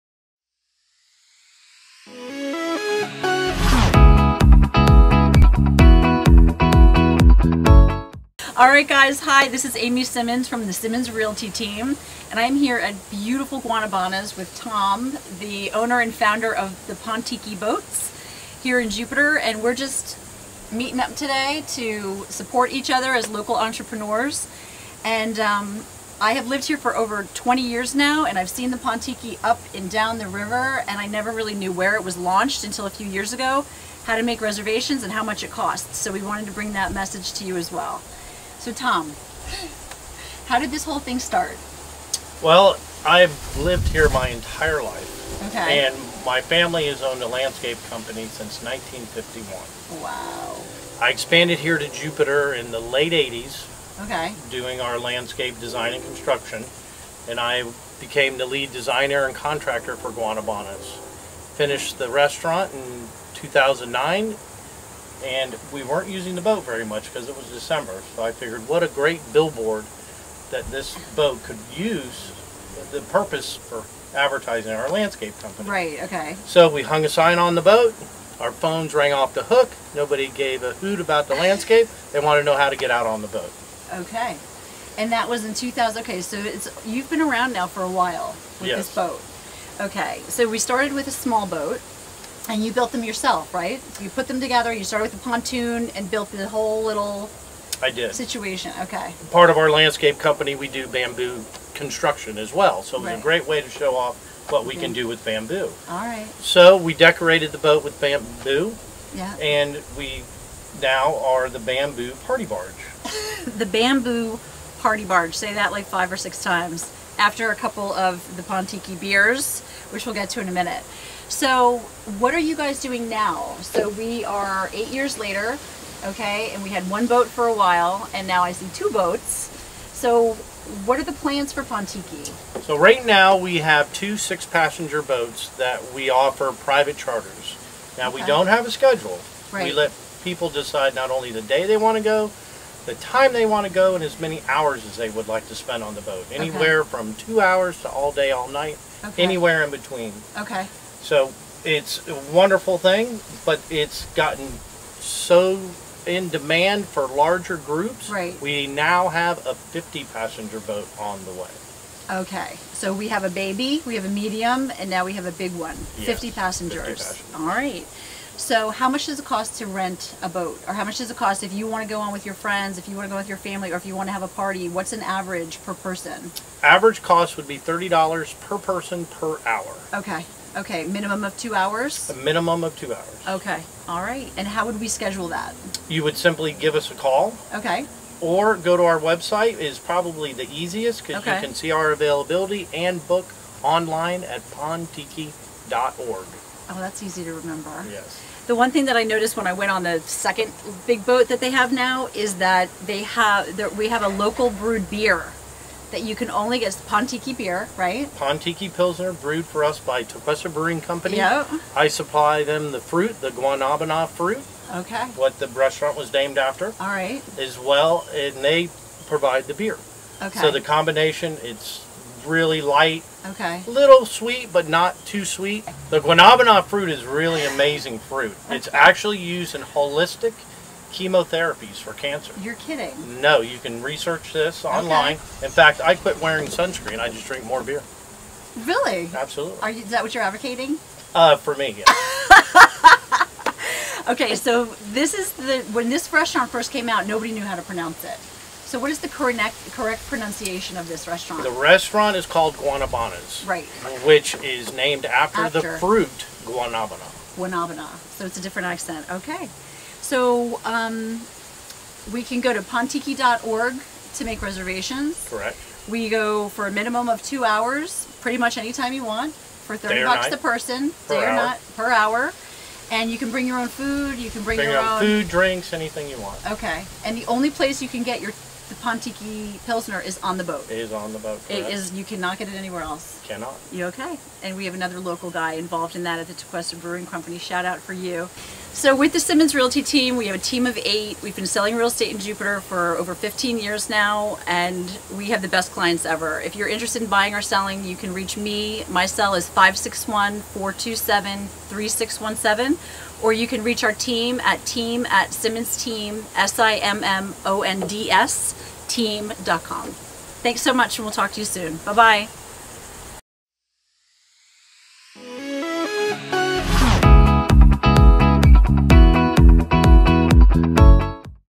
Jupiter Real Estate Agent:
Check out August's Entrepreneur of the Month video!  We chat with Captain Tom of the Pontiki!

Buying a home?

The Simmonds Team of Keller Williams